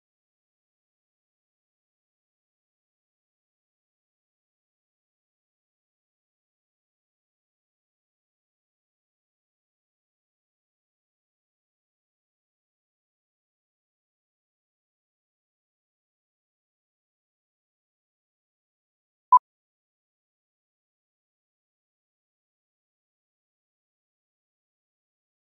Farm Animals 07 (Chicken) / Free Stock Footage
Description:
Footage of a chicken out in the wild.